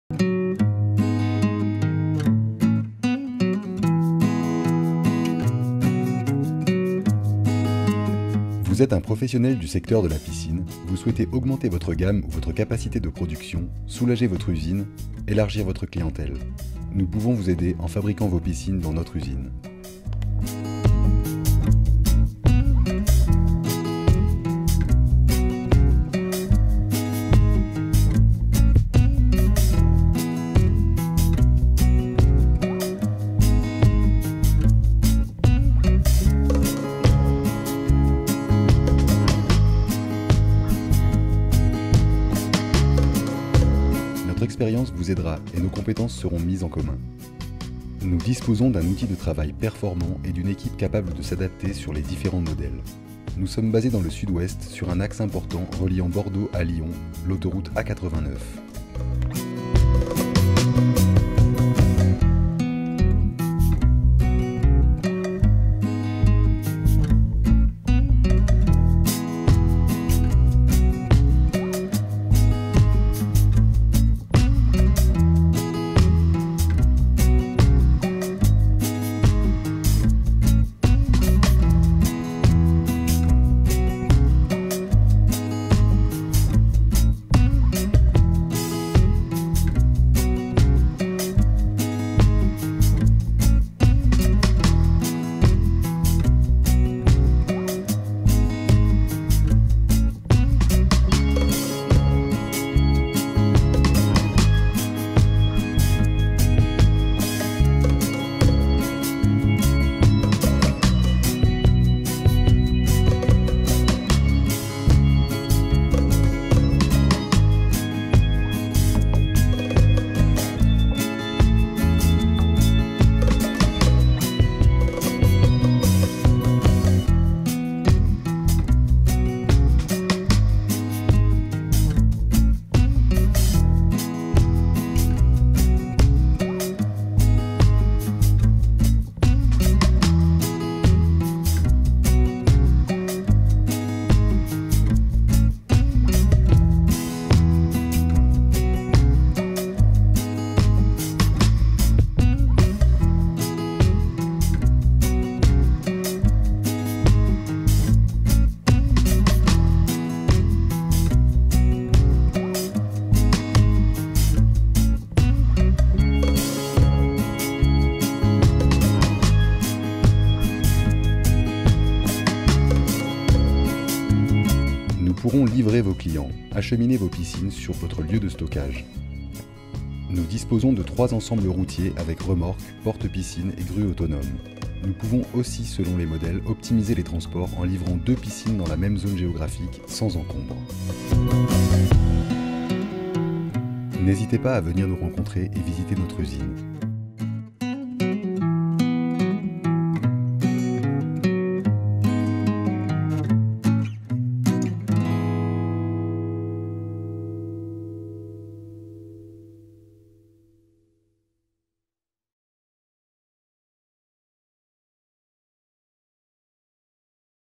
Concepteur de piscines - les Lèches : SARL COMPOSITE INDUSTRIE DU PERIGORD
Vous êtes un professionnel du secteur de la piscine, vous souhaitez augmenter votre gamme ou votre capacité de production, soulager votre usine, élargir votre clientèle.

Nous pouvons vous aider en fabriquant vos piscines dans notre usine
Nous pourrons livrer vos clients, acheminer vos piscines sur votre lieu de stockage.

Notre expérience vous aidera et nos compétences seront mise en commun.
Nous disposons d’un outil de travail performant et d’une équipe capable de s’adapter sur les différents modèles.

Nous sommes basés dans le Sud-Ouest sur un axe important reliant Bordeaux à Lyon ; l’autoroute A 89.
Nous disposons de 3 ensembles routiers avec remorque porte piscine et grue autonome, nous pouvons aussi selon les modèles optimiser les transports en livrant 2 piscines dans la même zone géographique sans encombre.

N’hésitez pas, venez nous rencontrer et visiter notre usine, consulter nous :

COMPOSITE INDUSTRIE DU PERIGORD
ZAE LES GRAULES
24400 LES LECHES